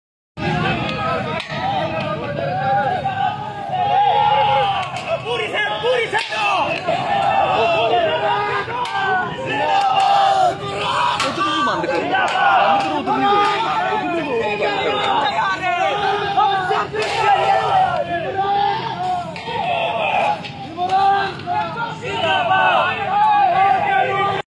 BREAKING: Former Pakistani PM Imran Khan arrested in Islamabad
Welcome to the world of Gold CreAtor shorts. This video is for funny to smile your faces